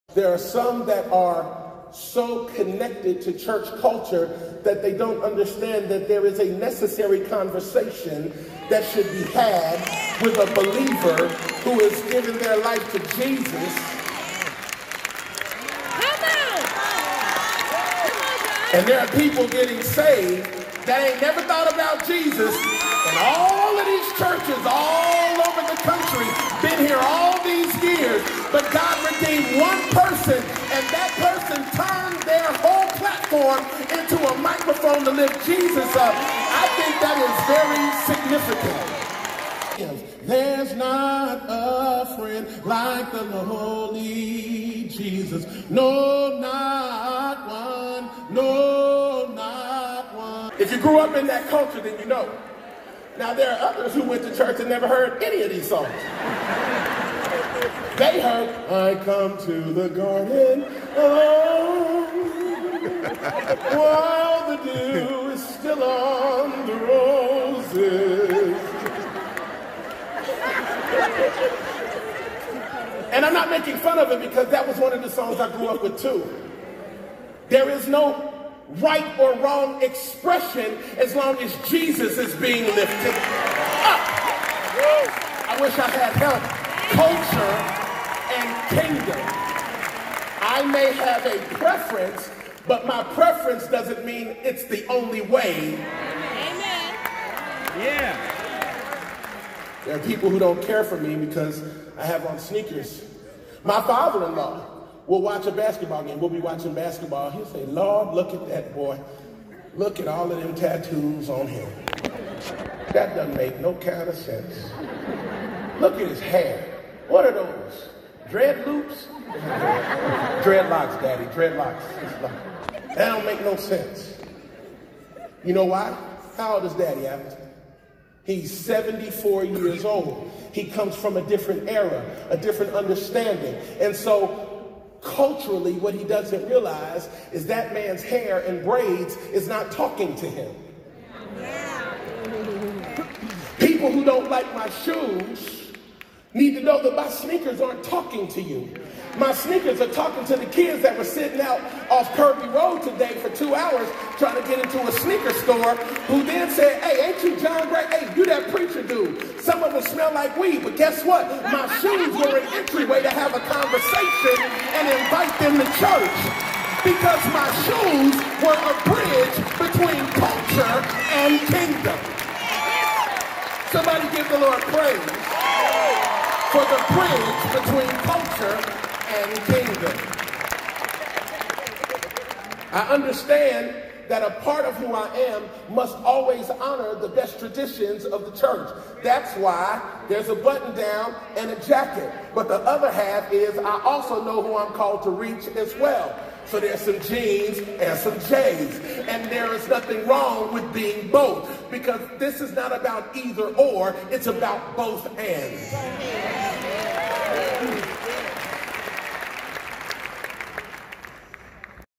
JOHN GRAY @LAKEWOOD. (CULTURE &KINGDOM)
Since My Wife been here we've been attending Lakewood church & it's been a Blessing she always listened to Joel Osteen when she was in the Philippines, I Promised her I would take her to meet him as well as some other Pastor's thx for watching
IF YOU SEE SOMEONE DOWN DONT BE AFRAID TO UPLIFTASPIRIT!